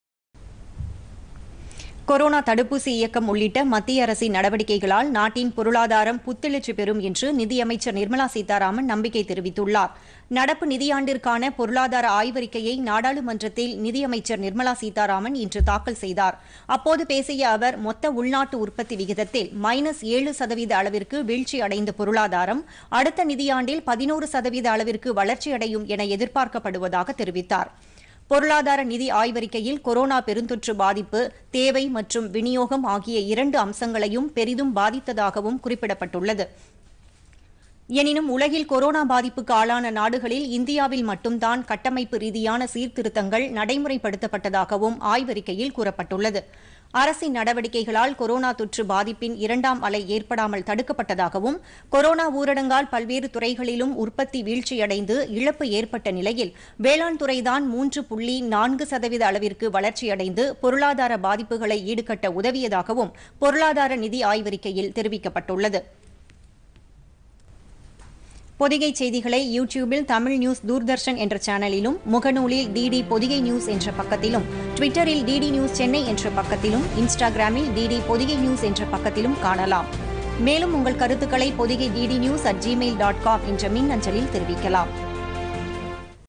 பொருளாதாரம் புத்தெழுச்சி பெறும்PodhigaiTamilNews
கொரோனா தடுப்பூசி இயக்கம் உள்ளிட்ட அரசின் நடவடிக்கைகளால் நாட்டின் பொருளாதாரம் புத்தெழுச்சி பெறும் - பொருளாதார ஆய்வறிக்கையில் நிதியமைச்சர் நிர்மலா சீதாராமன் நம்பிக்கை.